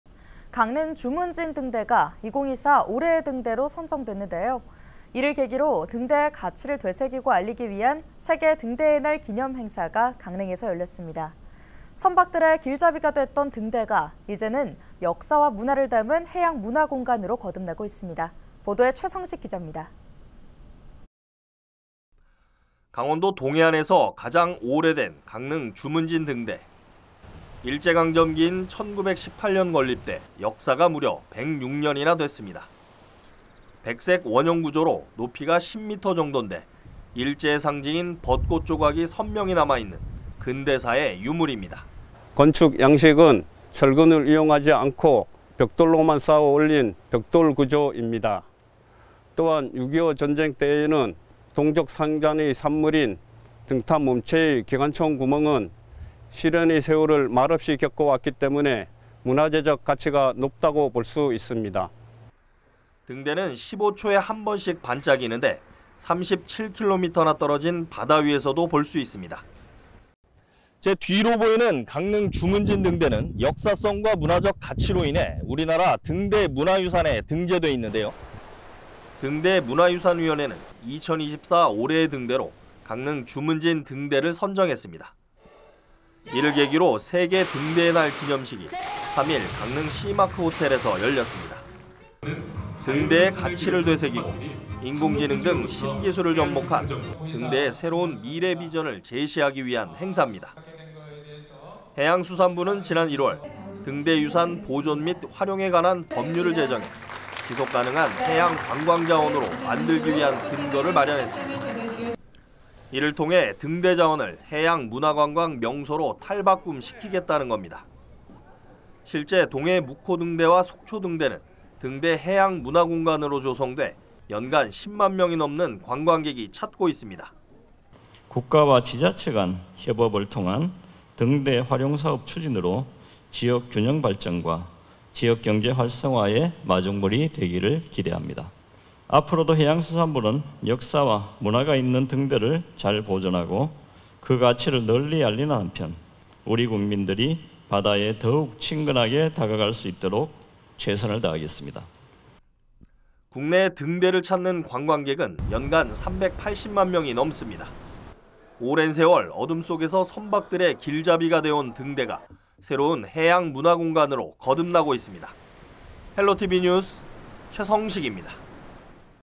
어둠 밝혀온 등대, 해양문화공간으로…
강릉 주문진등대가 2024 올해의 등대로 선정됐는데요.
이를 계기로
등대의 가치를 되새기고 알리기 위한 세계등대의 날 기념 행사가 강릉에서 열렸습니다.
선박들의 길잡이가 됐던 등대가 이제는 역사와 문화를 담은
해양문화공간으로 거듭나고 있습니다.

📌 카카오톡 ID : 헬로tv뉴스